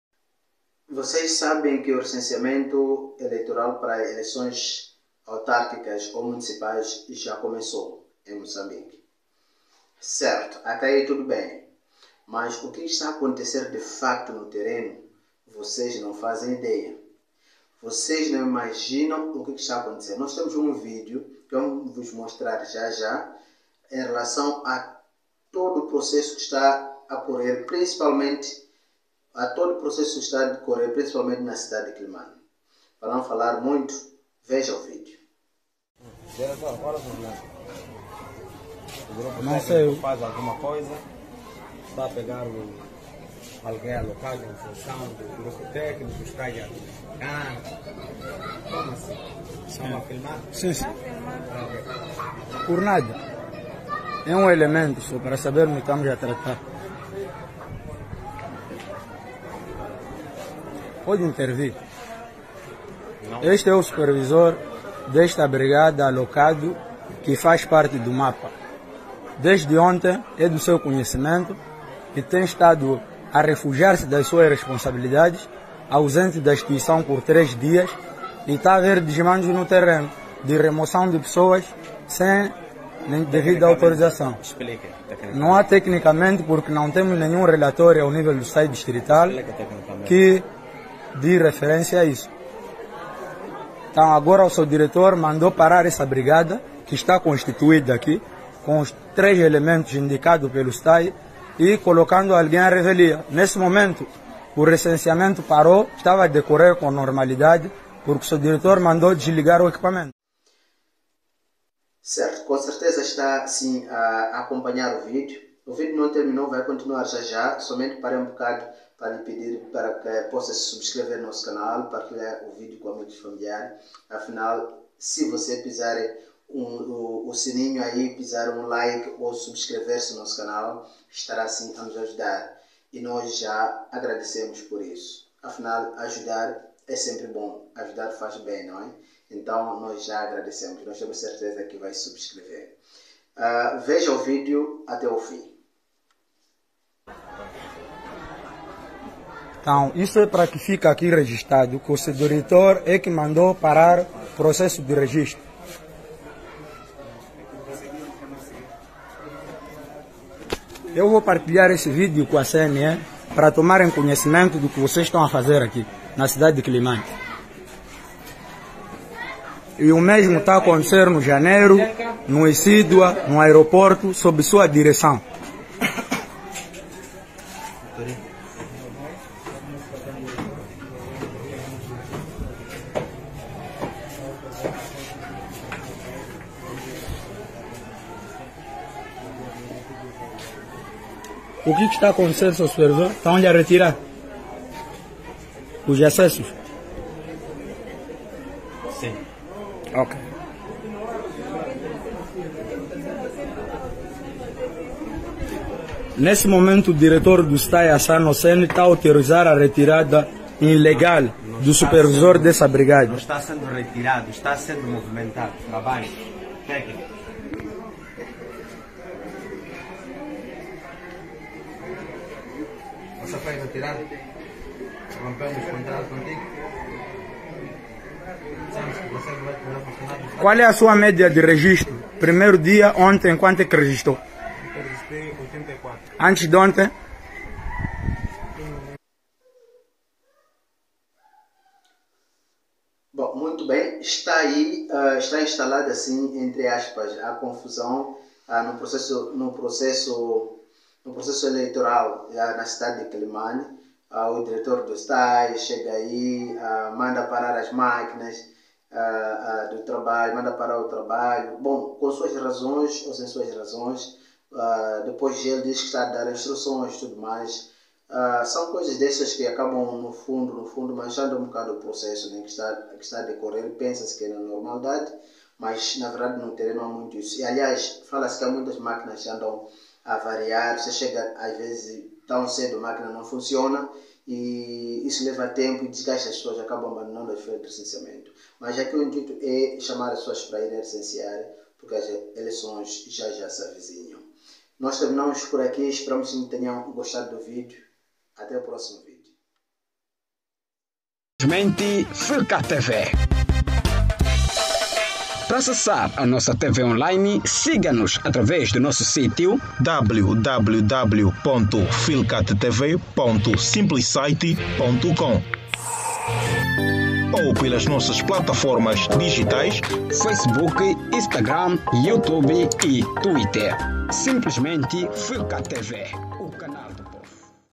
INÉDITO! DIRECTOR DO STAE EM QUELIMANE PARALIZA RECENSEAMENTØ ELEITORAL...
Veja no vídeo de hoje como é que o director do stae agiu com os brigadistas...